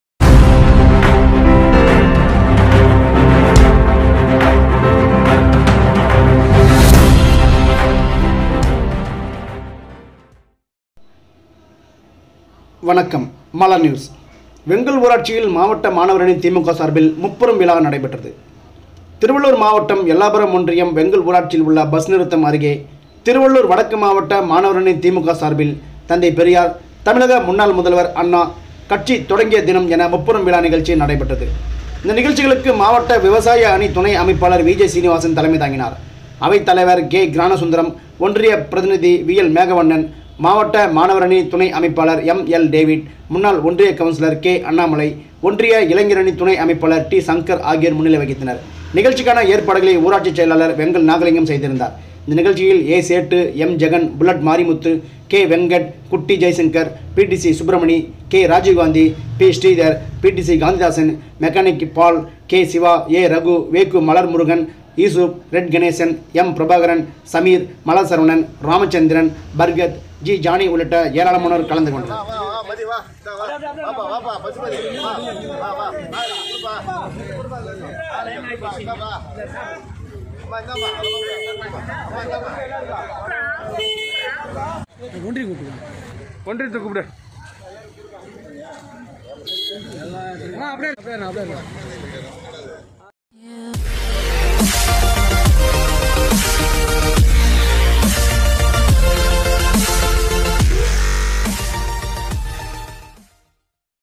எல்லாபுரம் ஒன்றியம் வெங்கல் ஊராட்சியில் மாவட்ட மாணவரணி திமுக சார்பில் முப்பெரும் விழா கொண்டாட்டம்
திருவள்ளூர் மாவட்டம், எல்லாபுரம் ஒன்றியம், வெங்கல் ஊராட்சியில் உள்ள பஸ் நிறுத்தம் அருகே திருவள்ளூர் வடக்கு மாவட்ட
மாணவரணி
திமுக சார்பில் தந்தை பெரியார்,தமிழக முன்னாள் முதல்வர் அண்ணா,கட்சி தொடங்கிய தினம் என முப்பெரும் விழா நிகழ்ச்சி நடைபெற்றது.இந்நிகழ்ச்சிகளுக்கு,மாவட்ட விவசாய அணி துணை அமைப்பாளர் வி.ஜே.சீனிவாசன்  தலைமை தாங்கினார். அவைத்தலைவர் கே. கிராணசுந்தரம்,ஒன்றிய பிரதிநிதி வி.எல்.மேகவண்ணன், மாவட்ட மாணவரணி துணை அமைப்பாளர் எம்.எல்.டேவிட்,முன்னாள் ஒன்றிய கவுன்சிலர் கே.அண்ணாமலை,ஒன்றிய இளைஞரணி துணை அமைப்பாளர் டி.சங்கர் ஆகியோர் முன்னிலை வகித்தனர்.நிகழ்ச்சிக்கான ஏற்பாடுகளை ஊராட்சி செயலாளர் வெங்கல் நாலிங்கம் செய்திருந்தார். இந்நிகழ்ச்சியில்,எ.சேட்டு,எம்.ஜெகன்,புல்லட்மாரிமுத்து,கே.வெங்கட்,
குட்டிஜெய்சங்கர்,பி.டி.சி.சுப்பிரமணி,கே.ராஜிவ்காந்தி, பி.ஶ்ரீதர்,பி.டி.சி.காந்திதாசன்,மெக்கானிக்பால்,
கே.சிவா,எ.ரகு, வெ.கு.மலர்முருகன்,ஈசூப்,ரெட்கணேசன், எம்.பிரபாகரன்,சமீர்,மலர்சரவணன்,ராமசந்திரன்,
பர்கத்,ஜி.ஜானி உள்ளிட்ட ஏராளமானோர் கலந்து கொண்டனர்.